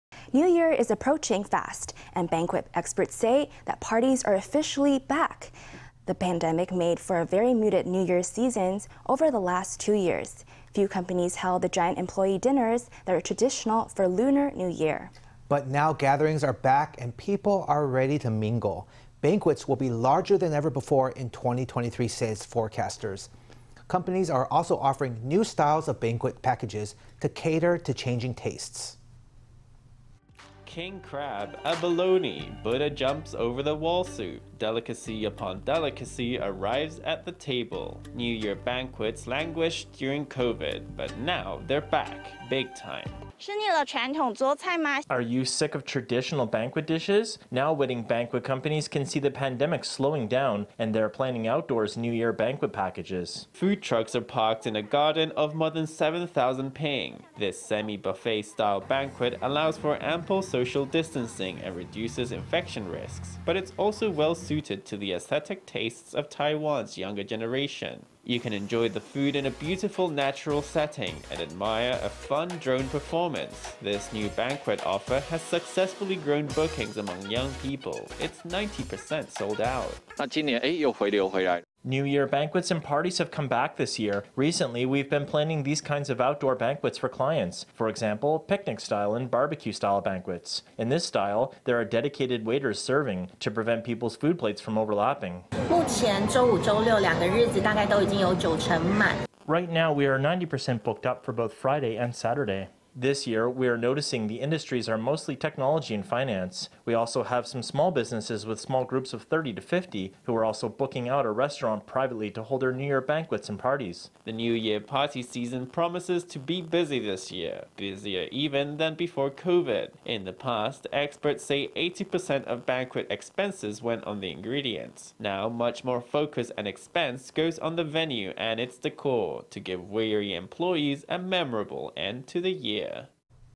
New Year banquets are back on the menu as COVID recedes into the past
New Year is approaching fast, and banquet experts say that parties are officially back. The pandemic made for very muted New Year seasons over the last two years. Few companies held the giant employee dinners that are traditional for Lunar New Year. But now gatherings are back, and people are ready to mingle. Banquets will be larger than ever before in 2023, say forecasters. Companies are also offering new styles of banquet packages, to cater to changing tastes.

King crab, abalone, buddha jumps over the wall soup… Delicacy upon delicacy arrives at the table. New Year banquets languished during COVID, but now they’re back – big time.

Tang Shih-ching
FTV reporter
Are you sick of traditional banquet dishes? Now wedding banquet companies can see the pandemic slowing down, and they’re planning outdoors New Year banquet packages.

Food trucks are parked in a garden of more than 7,000 ping. This semi-buffet style banquet allows for ample social distancing and reduces infection risks, but it’s also well suited to the aesthetic tastes of Taiwan’s younger generation.

You can enjoy the food in a beautiful natural setting, and admire a fun drone performance. This new banquet offer has successfully grown bookings among young people. It’s 90% sold out.

Hsieh Wei-tien
Wedding banquet industry
New Year banquets and parties have come back this year. Recently we’ve been planning these kinds of outdoors banquets for clients, for example, picnic style and barbecue style banquets. In this style, there are dedicated waiters serving, to prevent people’s food plates from overlapping.

Shao Chih-chi
Hotel PR manager
Right now, we are 90% booked up for both Friday and Saturday. This year we’re noticing the industries are mostly technology and finance. We also have some small businesses with small groups of 30 to 50, who are also booking out a restaurant privately to hold their New Year banquets and parties.

The New Year party season promises to be busy this year – busier even than before COVID. In the past, experts say 80% of banquet expenses went on the ingredients. Now much more focus and expense goes on the venue and its décor, to give weary employees a memorable end to the year.